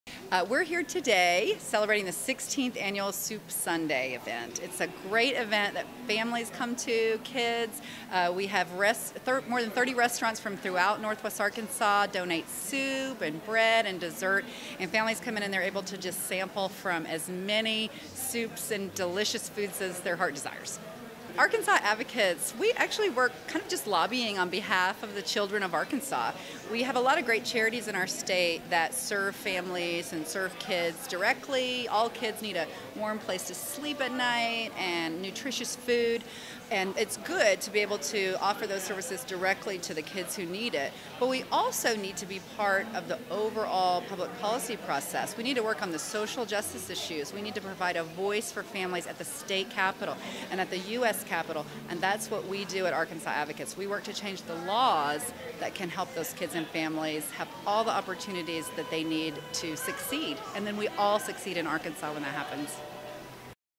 Laura Kellams AACF Soup Sunday Interview
Laura Kellams is the N.W. Arkansas director of the Arkansas Advocates for Children and Families - a respected and effective group that lobbies on behalf of the residents of Arkansas. The recent "Soup Sunday" event was held to raise awareness and funds for A.A.C.F.  I spoke with Laura about the group and the Soup Sunday event.